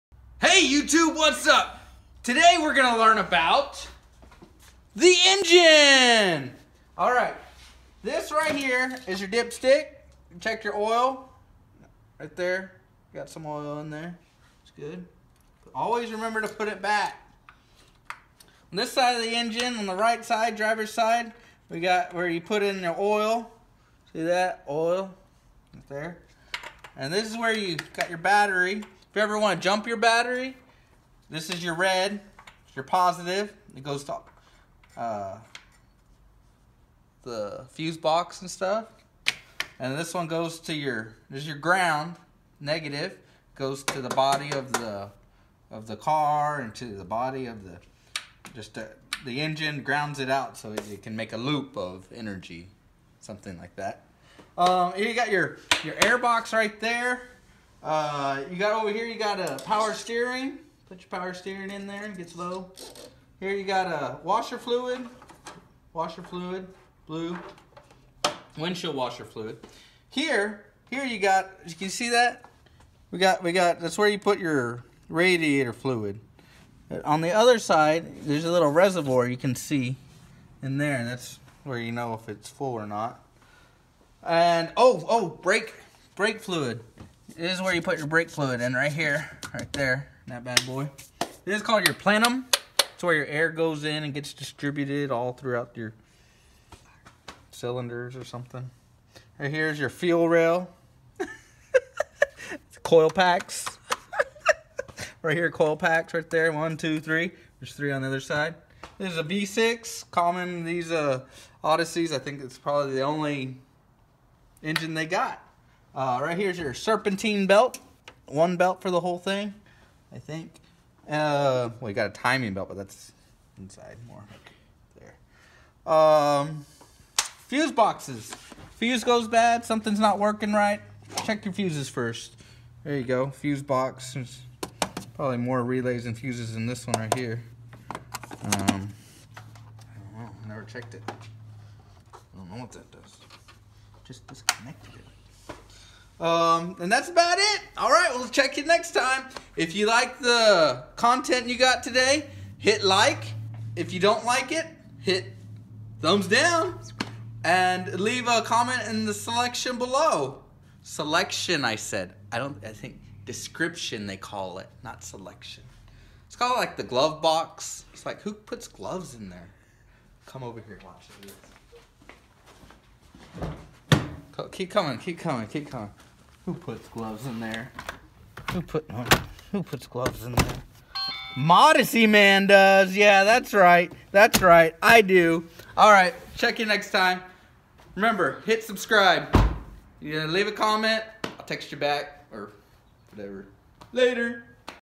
What’s under my Hood? 2006 Honda Odyssey
Looking at the engine bay of my Honda Odessey. What things are and stuff.